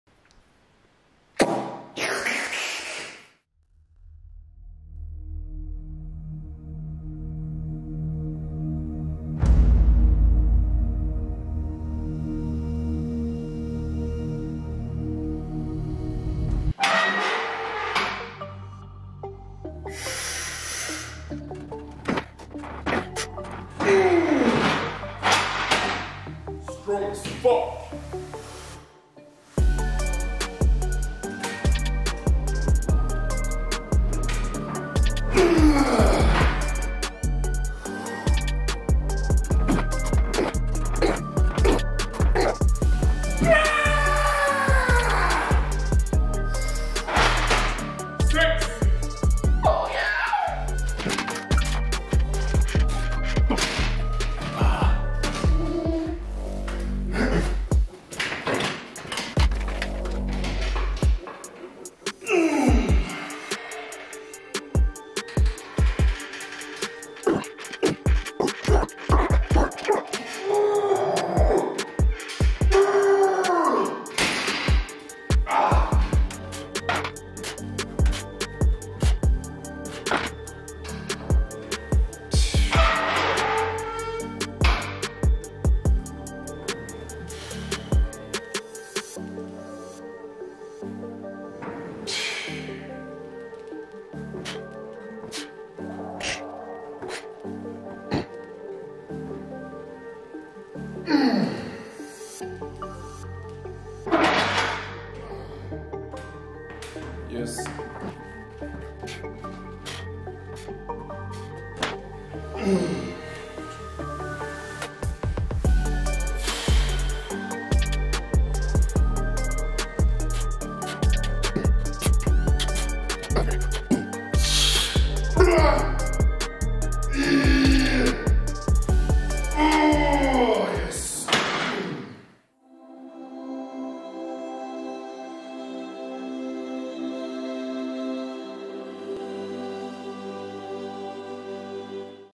The Right Kind of Pain!
🗓️ Tue 6 Jun 🗓️
1: Hack Squat
2: Leg Curl
3: Calf Press
4: Tibi Press
🟢 Coffee ☕️
🟢 Meat & Eggs 🥩🥚

🔴 Low Carb
🔴 Low Plant

💪 Arm:                  40cm | 15in
🤳 Forearm:           35cm | 13in
🦵 Leg:                   64cm | 25in
🐄 Calf:                   42cm | 16in